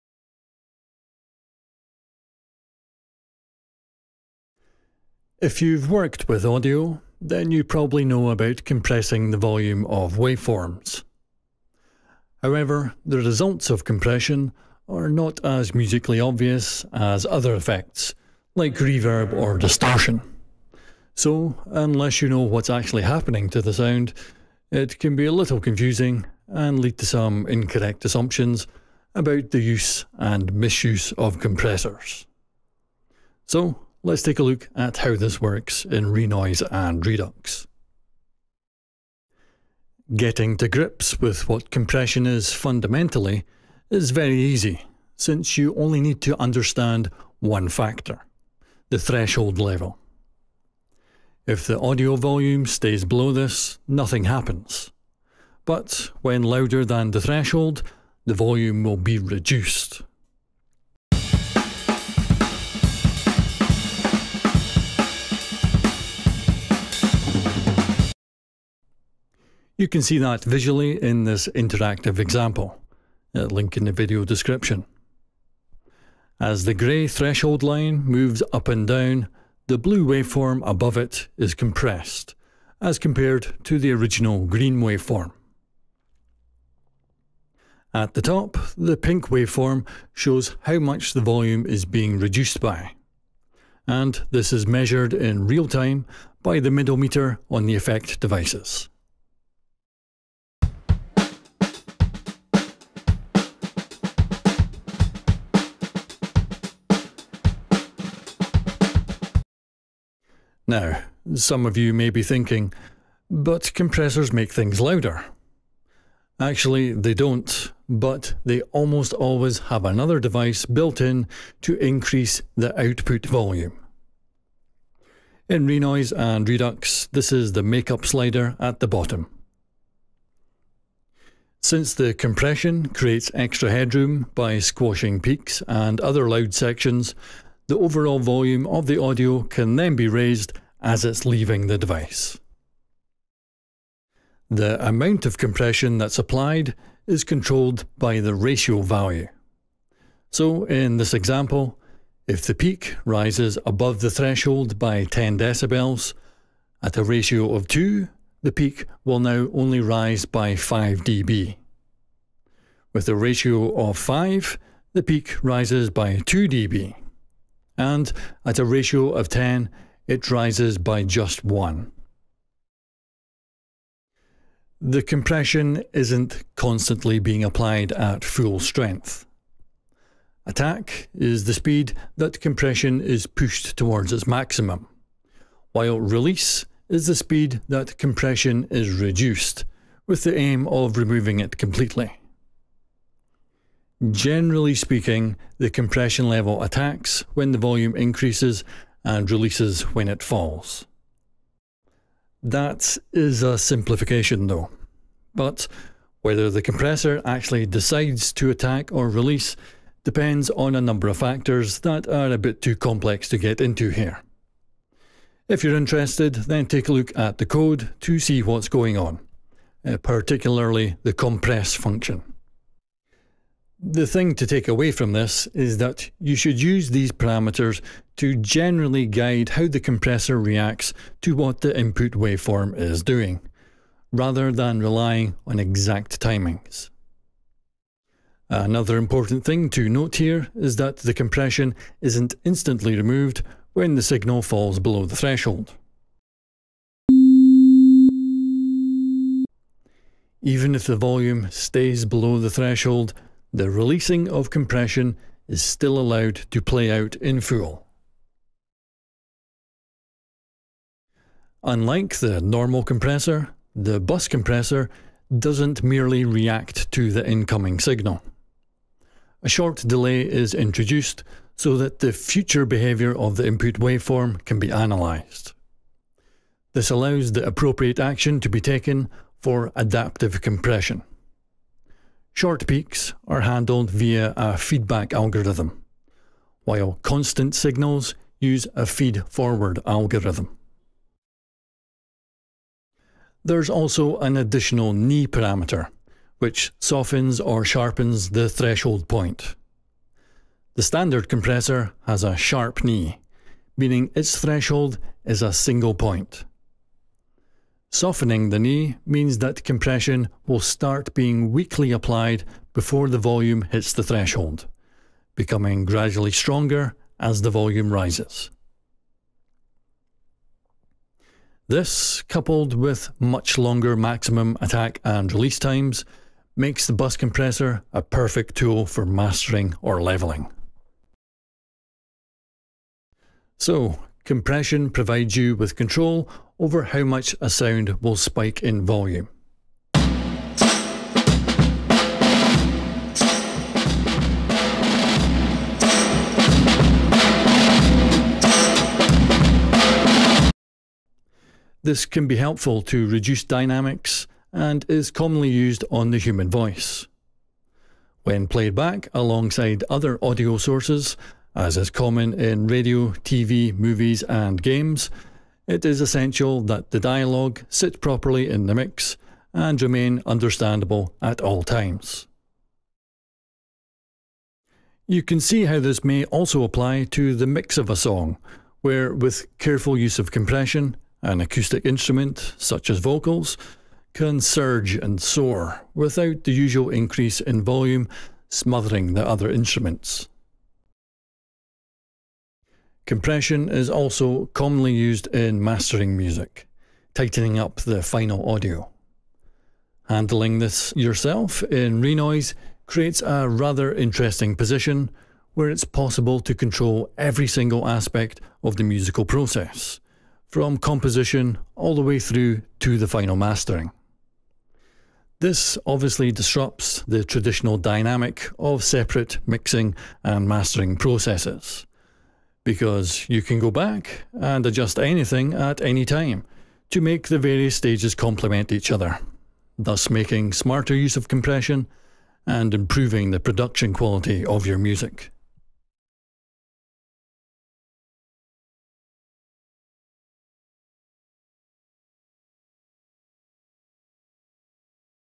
Compression
The results of compression are not as musically obvious as other effects like reverb & distortion, which can lead to some incorrect assumptions about the use and misuse of compressors. So let's take a look at how this works in Renoise & Redux.

0:00 - Intro
0:39 - The Basics - Threshold & Ratio
2:54 - Variable Compression - Attack & Release
4:21 - Bus Compressor
5:33 - Using Compression